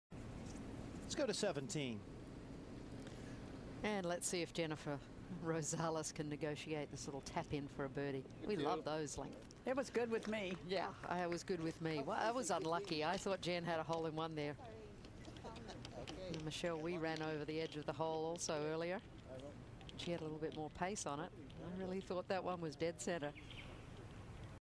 Jennifer Rosales LPGA Micro Bell LPGA Championship
Tap for birdie by Jennifer Rosales at 17th Hole Par 3 May 14, 2010